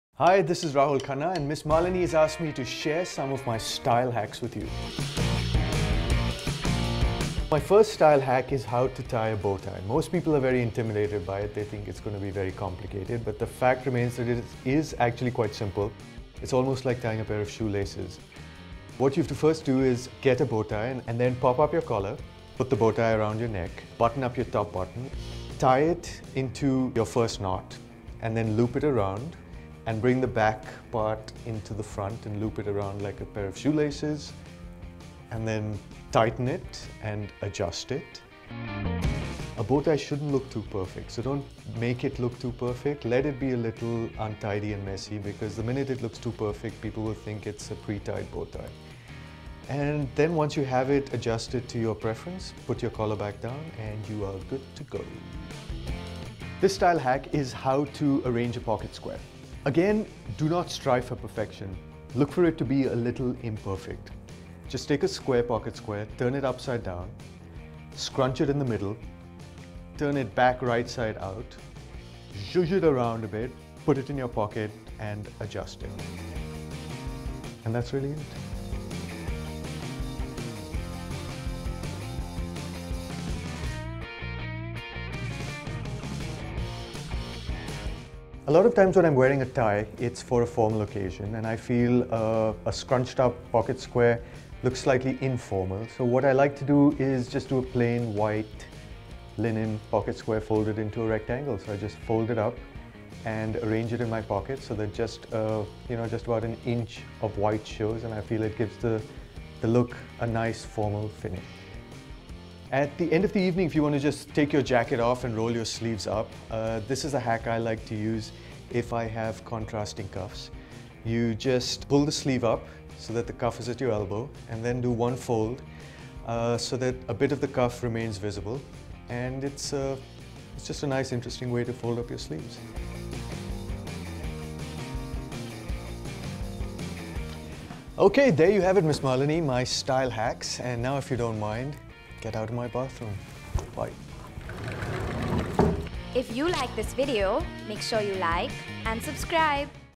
Rahul Khanna's Style Tips For Men
Rahul Khanna's too sexy for his shirt, but when he does put on some clothes, he's stylish as hell! So we asked him to give all the men out there some great tips and tricks. Press play and here's hoping we all find our own Rahul Khannas!